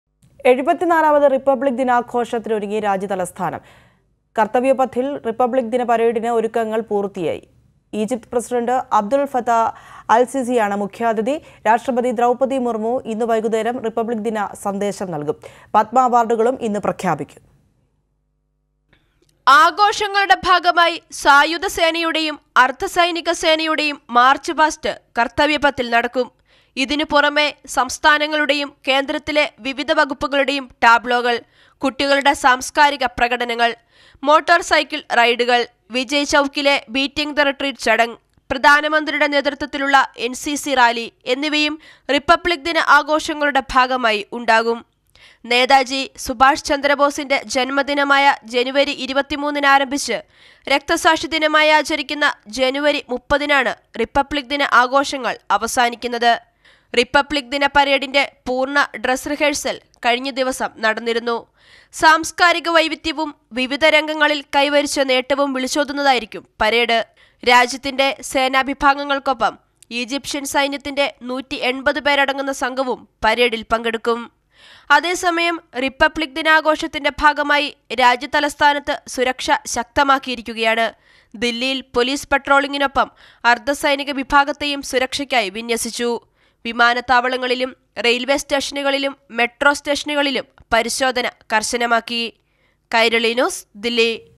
74 മത് റിപ്പബ്ലിക്ക് ദിനാഘോഷത്തിനൊരങ്ങി രാജ്യതലസ്ഥാനം; പത്മ അവാര്‍ഡുകൾ ഇന്ന് പ്രഖ്യാപിക്കും.
കര്‍തവ്യപഥില്‍ റിപ്പബ്ലിക്ക് ദിനപരേഡിന് ഒരുക്കങ്ങള്‍ പൂര്‍ത്തിയായി. ഈജിപ്ത് പ്രസിഡന്റ് അബ്ദുള്‍ ഫത്താ അല്‍സിസിയാണ് മുഖ്യാഥിതി. രാഷ്ട്രപതി ദ്രൗപതി മുര്‍മ്മു ഇന്ന് വൈകുന്നേരം റിപ്പബ്ബിക്ക് ദിന സന്ദേശം നല്‍കും. പത്മ അവാര്‍ഡുകളും ഇന്ന് പ്രഖ്യാപിക്കും.